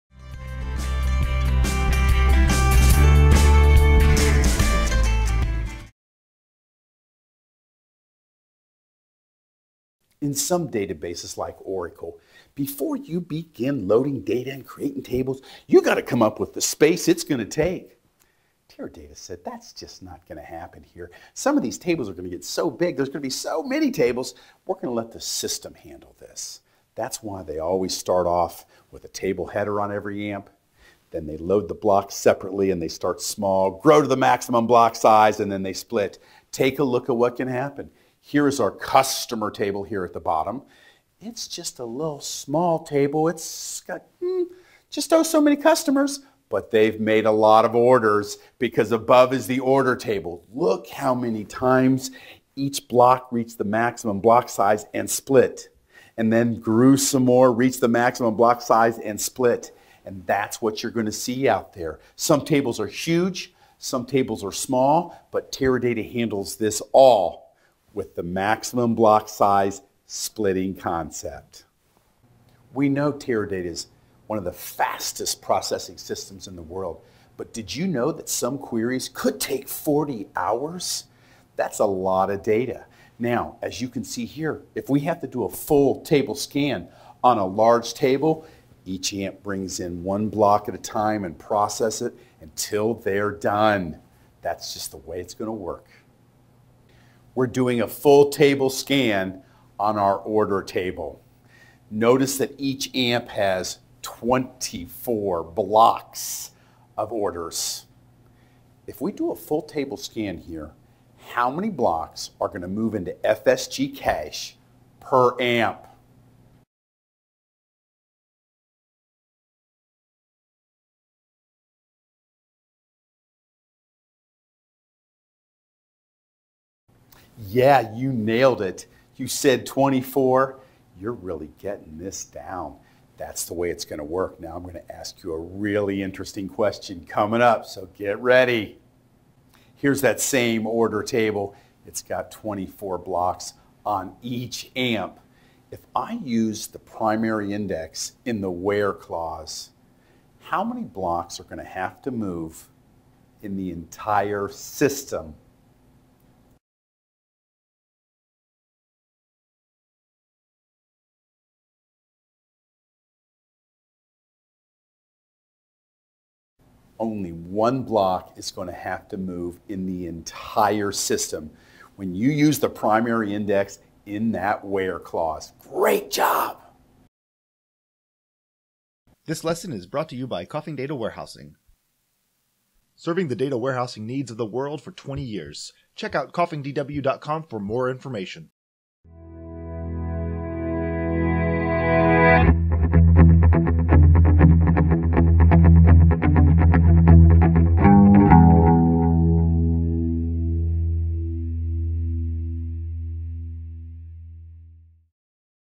Let's Learn Teradata Lesson 16: Teradata's Management of Data Blocks
Explore Teradata with Tera-Tom of Coffing Data Warehousing! In this lesson, learn how Teradata manages the data blocks in your system.